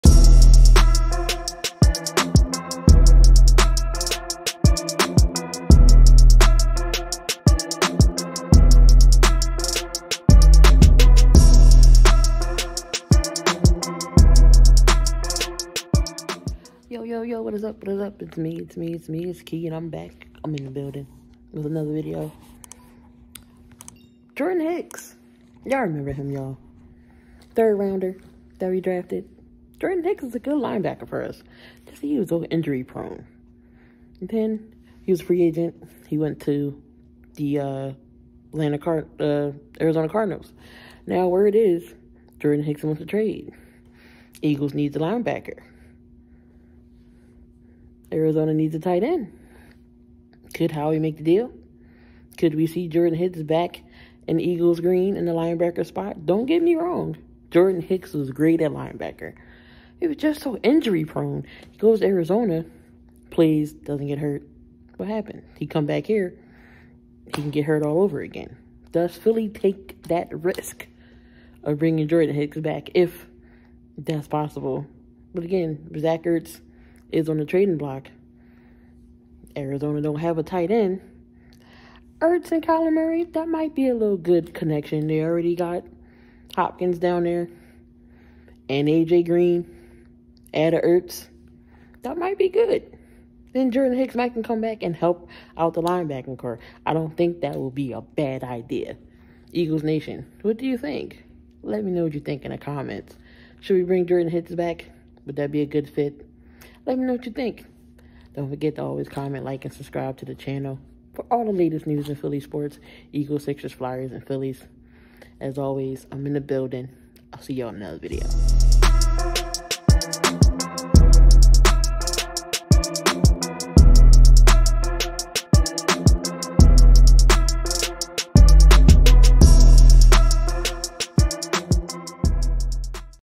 Could A Familiar Face Be back with the Birds
The Eagles continue to have a glaring need at linebacker but could add some depth if they wanted to make a trade to bring former draft pick Jordan Hicks back to Philly. Hicks spent the past two seasons with the Arizona Cardinals, but after drafting Tulsa LB Zaven Collins with their No. 16 overall pick in the 2021 NFL Draft, the team is reportedly letting Hicks find a new squad.